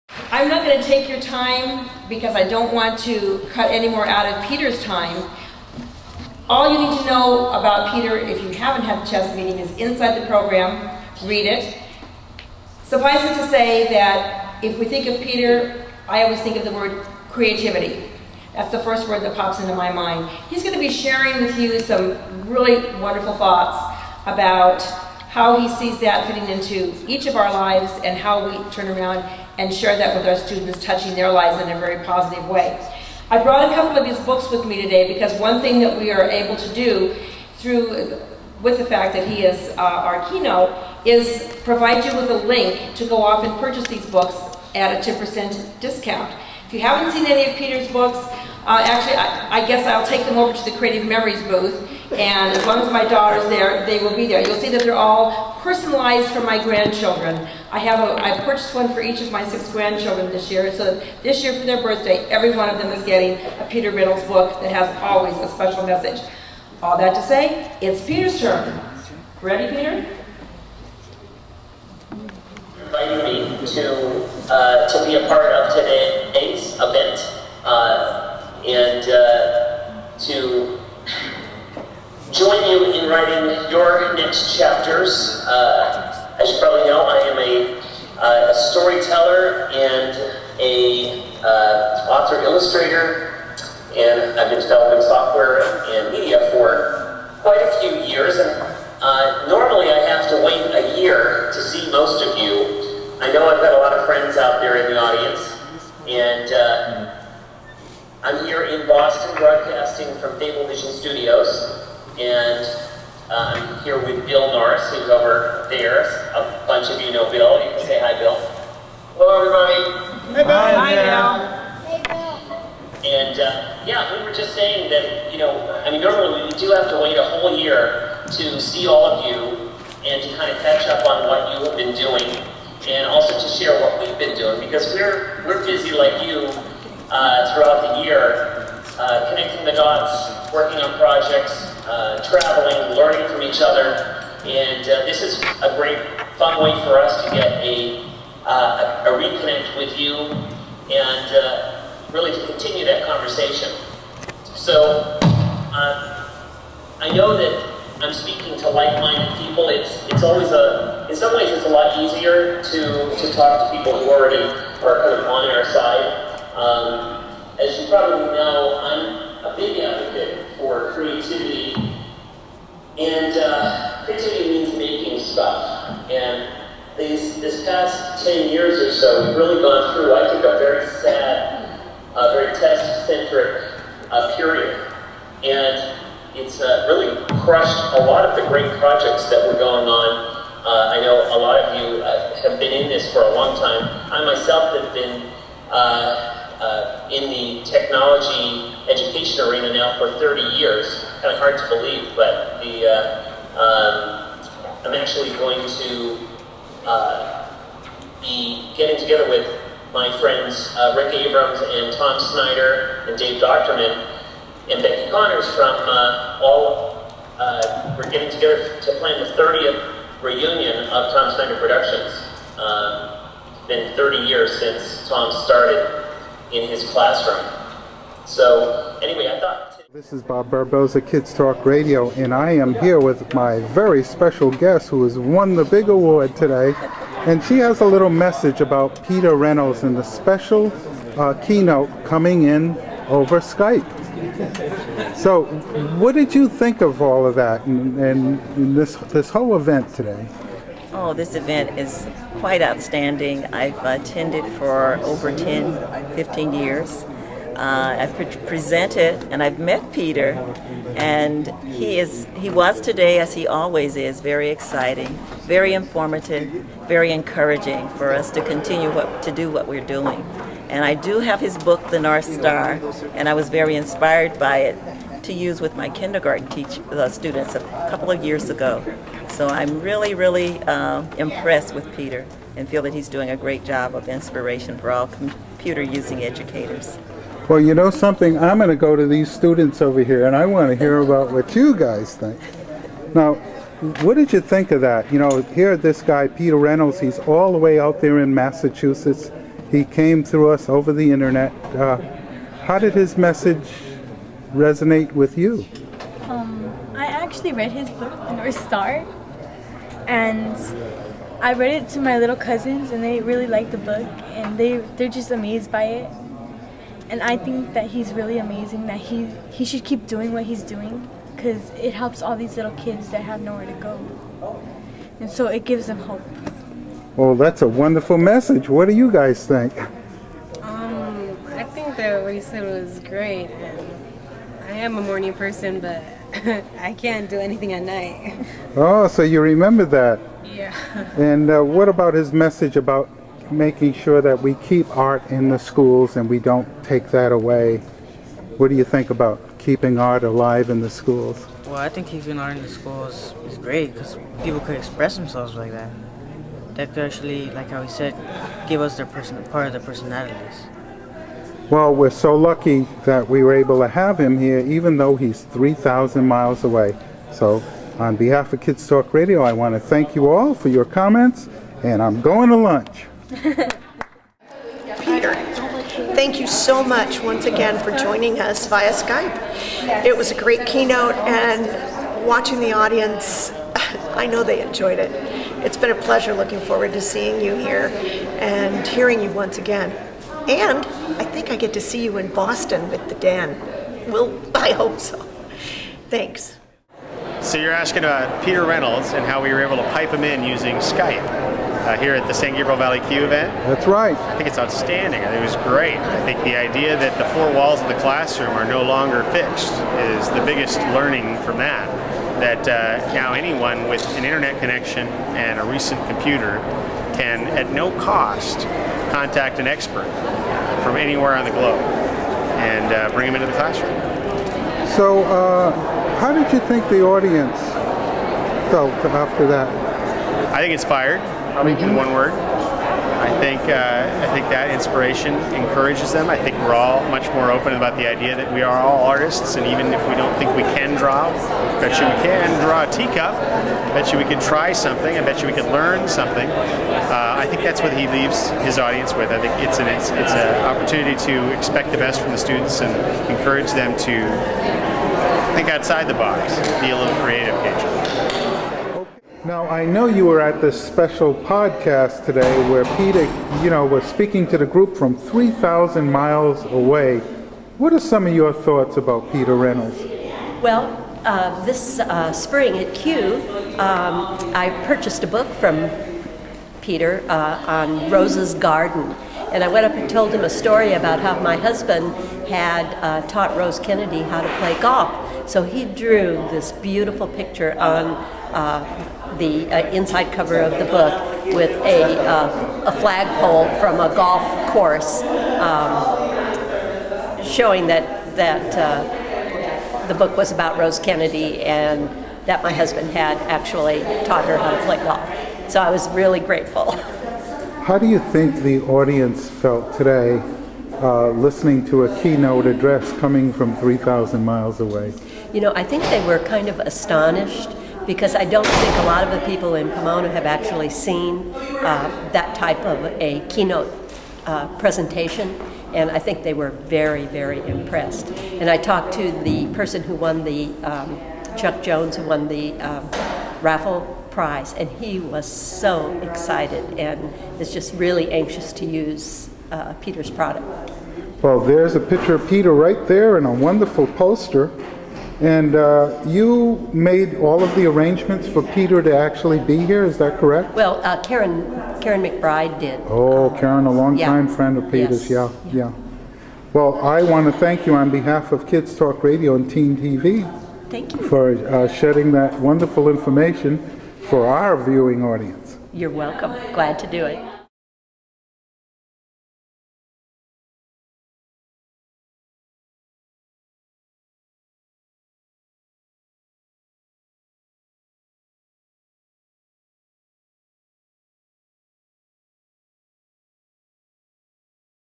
Peter Reynolds Keynote SGVCUE 2010.m4v
Peter Reynolds is a New York Times Best selling author and illustrator of dozens of childrens books in over 20 languages including Braille.  Peter lives in Dedham, Massachusetts and is founder and CEO of educational media company Fable Vision.

This Kids Talk Radio video Podcast is a sneak peek of Peters Keynote address titled Make Your Mark and See Where it Takes You.  Peter on Skype delivered the keynote address.  Peter was in his office at Fable Vision in Massachusetts.  We were all at the San Gabriel Valley Computer Using Educators 2010 Technology Fair in Pomona, California.

Peter is a great friend to Kids Talk Radio and has contributed drawings, animations, and educational software that are used by our student backpack journalist.

Send your questions or comments to:

Bob Barboza
Kids Talk Radio